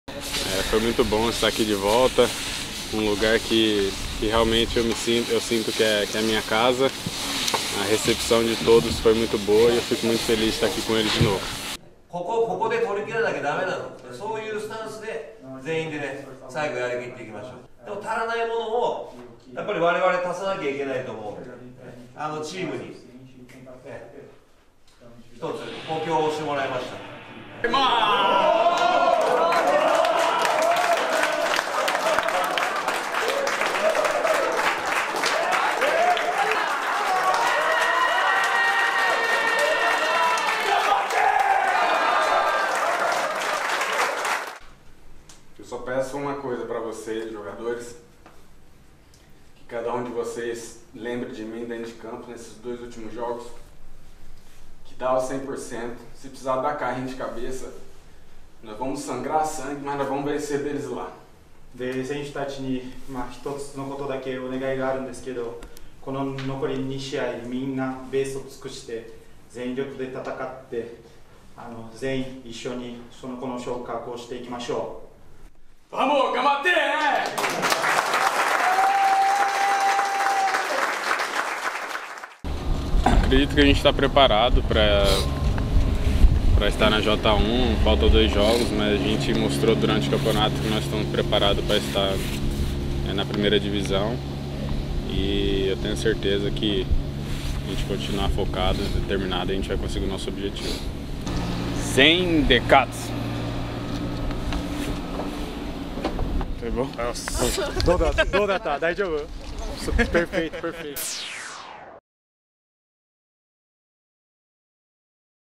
【FAGIGATE】グレイソンがサプライズで選手激励 「１年間の努力表現できれば昇格できる」
膝のけがの治療のため母国ブラジルに帰国していたファジアーノ岡山のＦＷグレイソン選手（２８）が２７日、練習拠点の政田サッカー場（岡山市）にサプライズで登場しました。J1昇格プレーオフに臨むチームへのエールやけがの具合などを聞きました。

※FAGIGATEは月額５５０円（税込み）。申し込みには山陽新聞ID（無料）の登録が必要です。詳しくは案内ページをご覧ください☟